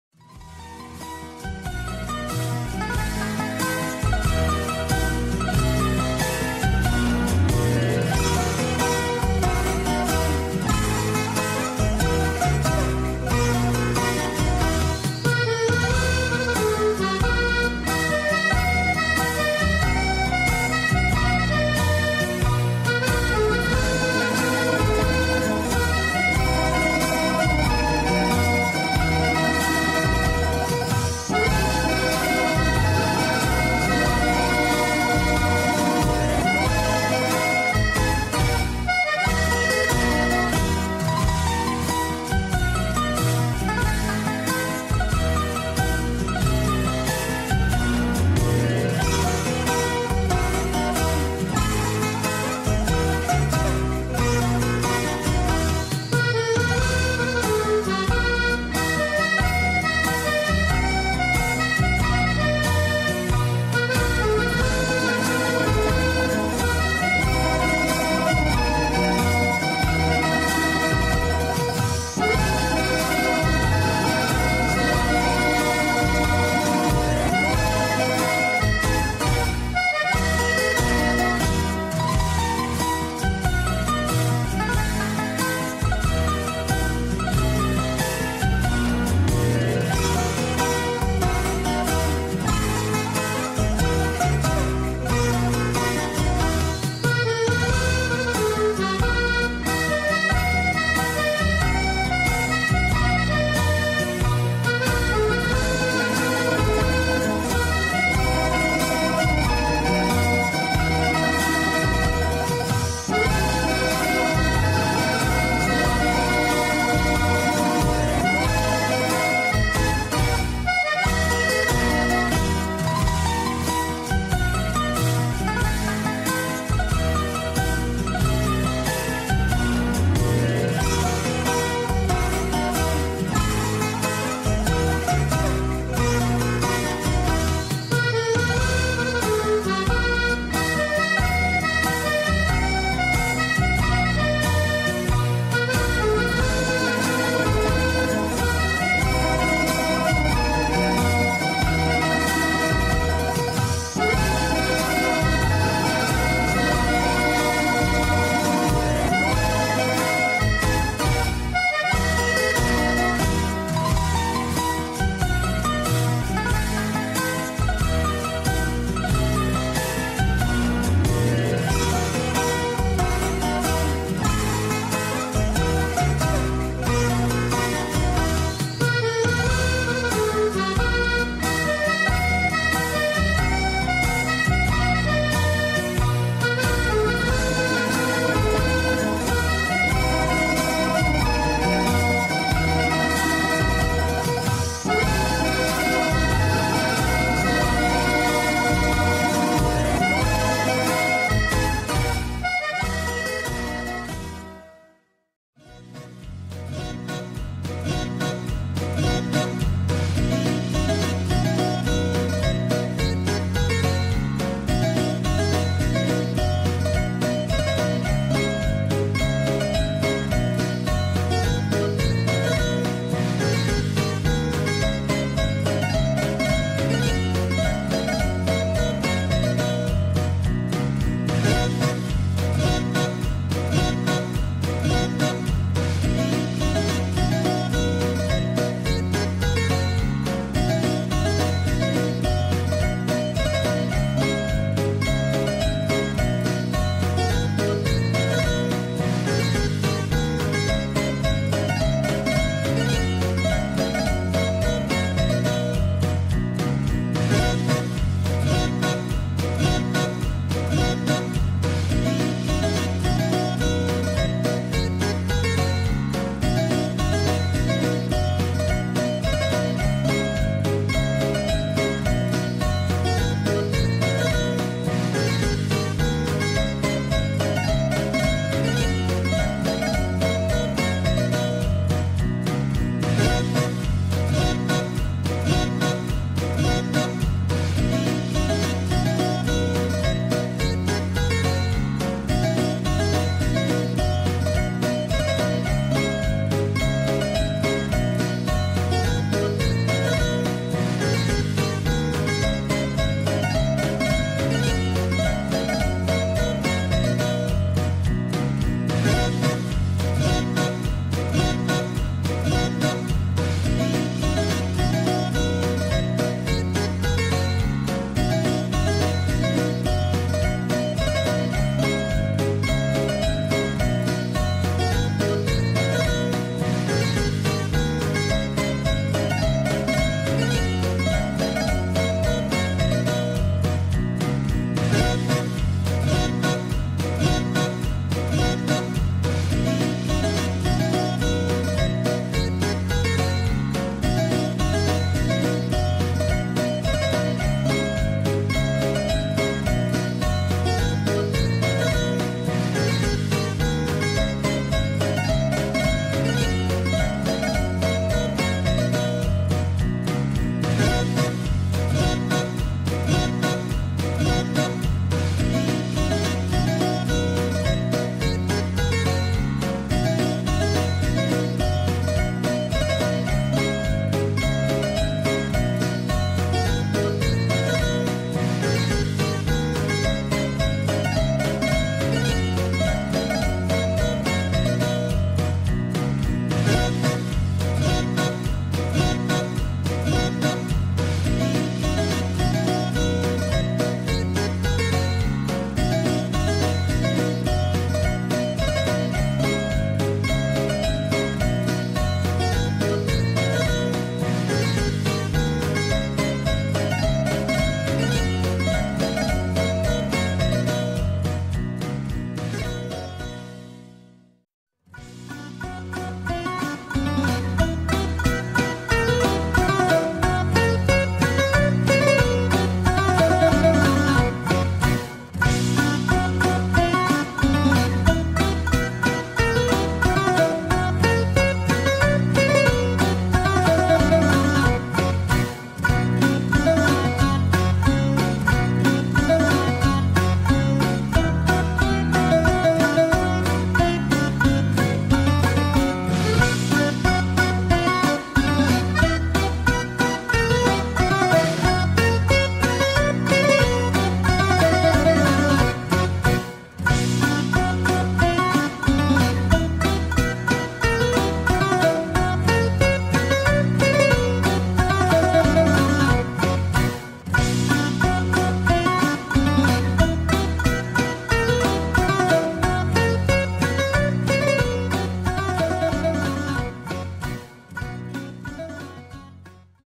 WITH MATSIGKOS SPEED BOAT TO LINDOS EVERY DAY
One day cruise with Matsigkos Speed Boat from Kolymbia to Lindos with stops at Red Sand Beach and Tsambika beach for swimming. Special crossing of the sea channel at Navarone canons.